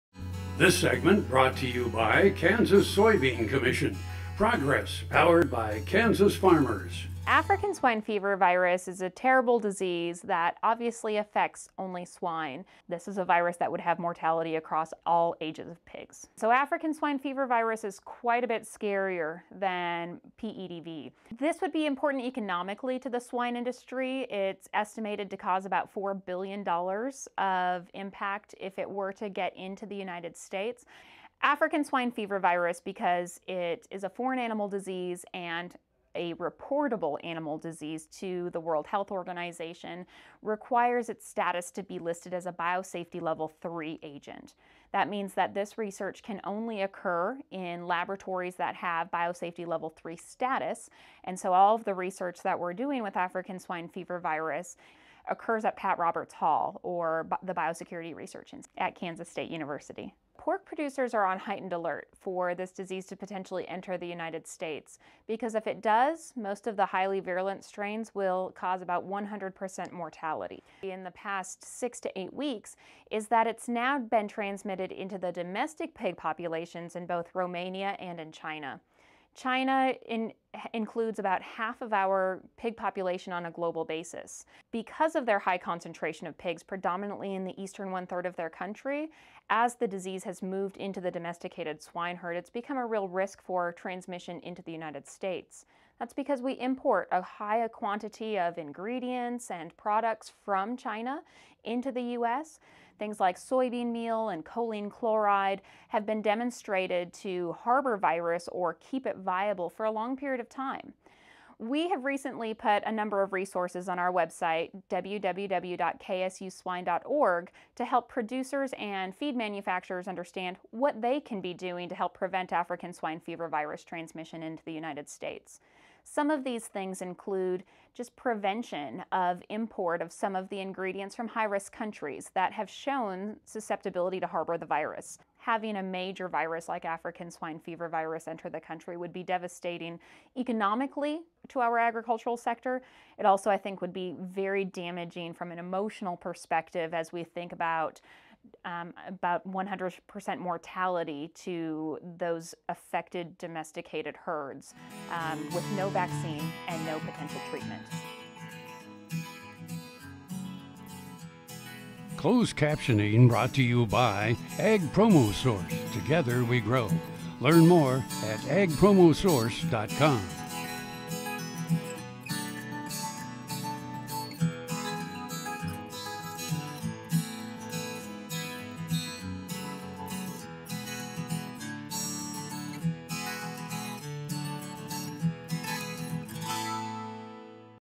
AGetc - African Swine Fever Virus January 03, 2019
Female Speaker: African swine fever virus is a terrible disease that obviously effects only swine. This is a virus that would have mortality across all ages of pigs. So African swine fever virus is quite a bit scarier than PEDV. This would be important economically to the swine industry.